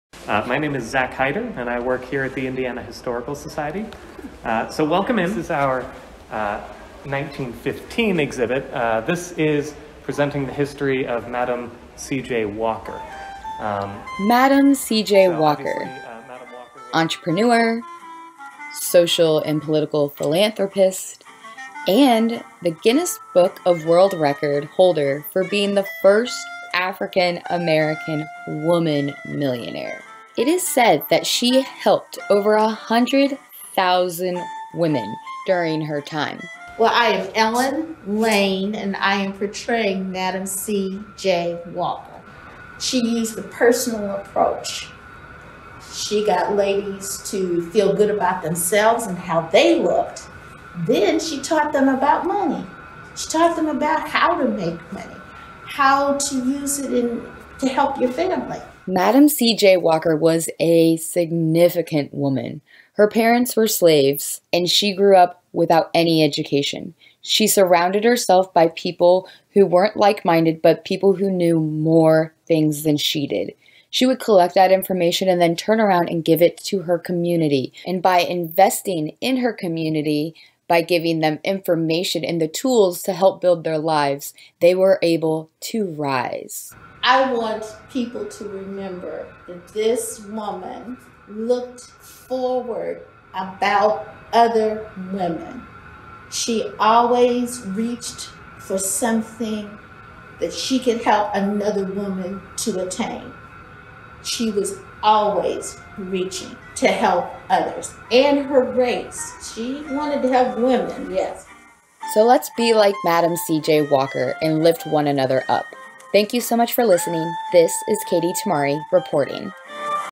Madam C.J. Walker and Women's History Month: An Interview with Zack Heider and Ellen Lane
Katie Tamarri interviewed Zack Heider and Ellen Lane at the Indiana Historical Society about Madam C. J. Walker and how she was the definition of a leader and entrepreneur. With last month being Black History Month and this month being Women's History Month, this interview helps show how Madam C. J. Walker was a perfect example of black excellence and a true pioneer for women’s rights.

Find more about us online here: thecampuscitizen.com